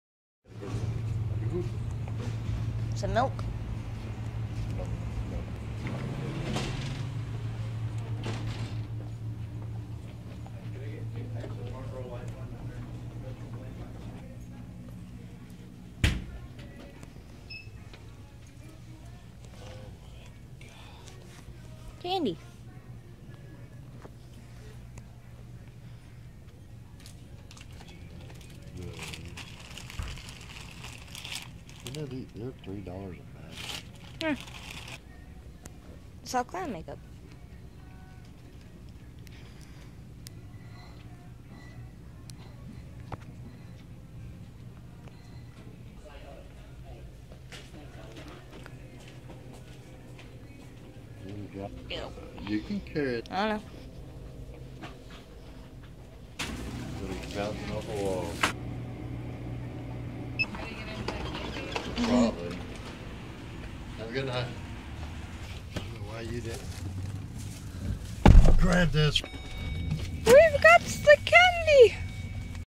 Candy trip at 8:47 PM
Lolol we went to Walgreen's and gotz candy and malk.